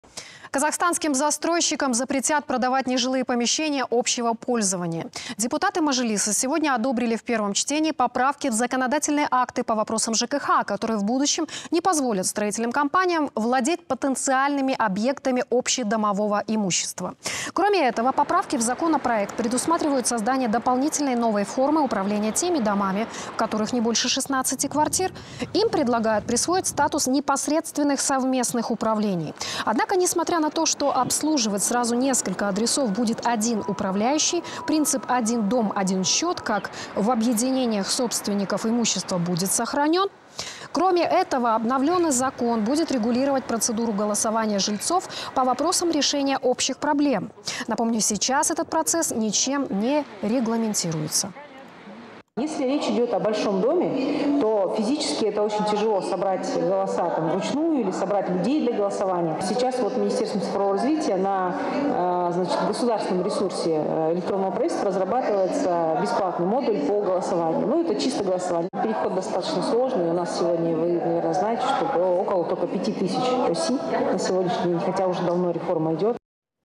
Для домов, в которых не более 16 квартир, появится новая форма управления
Казахстанским застройщикам запретят продавать нежилые помещения общего пользования. Депутаты Мажилиса сегодня одобрили в первом чтении поправки в законодательные акты по вопросам ЖКХ, которые в будущем не позволят строительным компаниям владеть потенциальными объектами общедомового имущества.